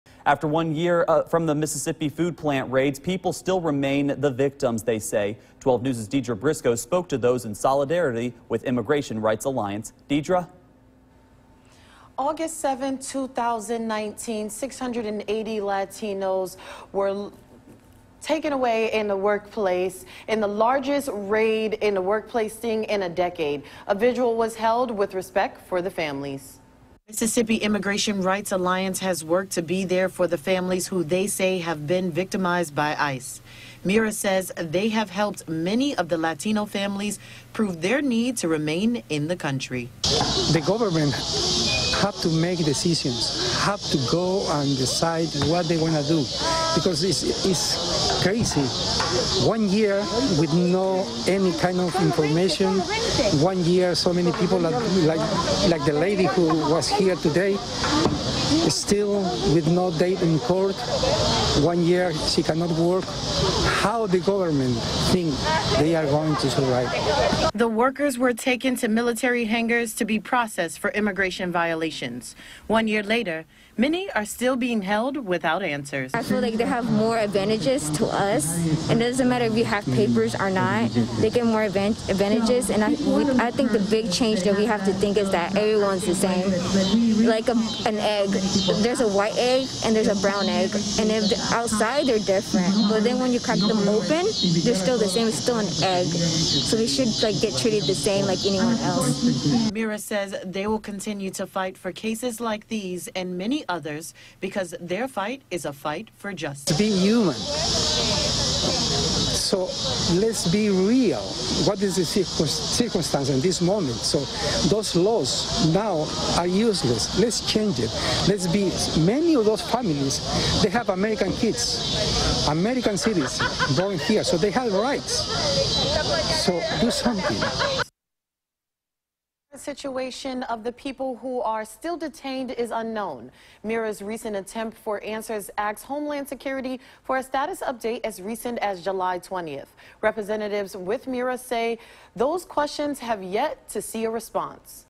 Immigration Rights Alliance holds solitary vigil for families still with no answers since 2019 raids
Mississippi Immigration Rights Alliance has worked to be there for the families who they say have been victimized by ICE. MIRA says they have helped many of the Latino families prove their need to remain in the country.